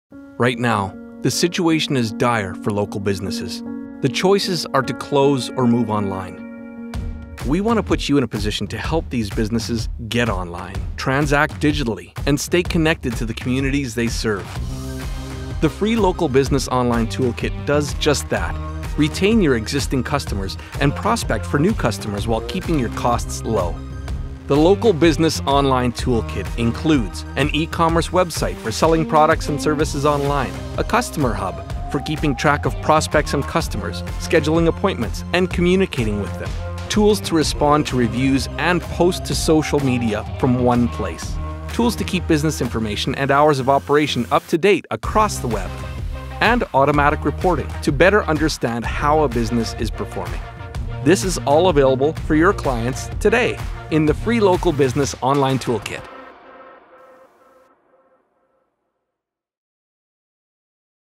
[Archived] Local Business Online Toolkit
When the pandemic hit, Vendasta was quick to respond. We pivoted and created this Local Business Online Toolkit to help businesses succeed online when they needed it the most. As we emerge from this unprecedented global challenge, the immediate need is subsiding. However, the importance of businesses to operate and operate well online will not go away, and neither will the tools offered by Vendasta in this video.

Original video description:
Local businesses are facing hard times right now. That’s why we’ve created a set of free tools for you to provide to them. The Online Toolkit includes e-commerce & online communication tools to keep businesses open by being able to sell their products online.